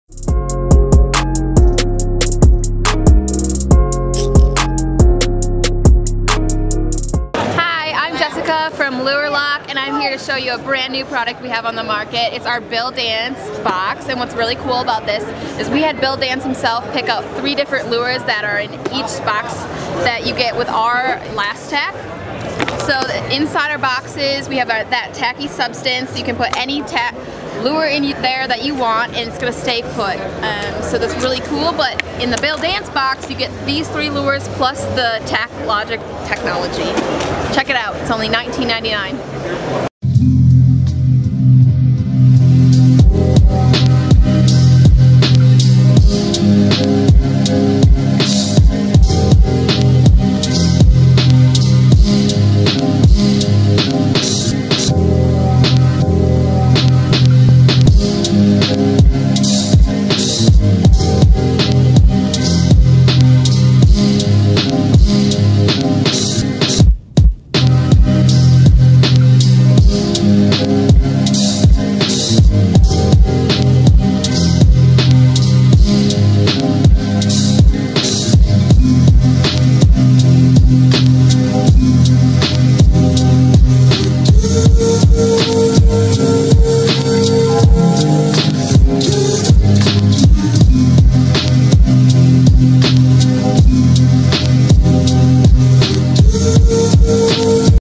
Lure Lock Bill Dance Tackle Box at 2019 Bassmaster Classic
Jessica Kriesel from Lure Lock discusses the new Lure Lock Bill Dance Tackle Box at the 2019 Bassmaster Classic on the Tennessee River in Knoxville, Tennessee. The Bassmaster Classic is bass fishing’s most prominent tournament featuring the top 50 anglers in the sport. The TackleDirect Production Crew was at the 2019 Classic filming all of the hottest and latest products from bass fishing's largest tournament of the year. Don't forget to check out our Bass specific product page “ for bass fishing tackle and gear.

We're Social!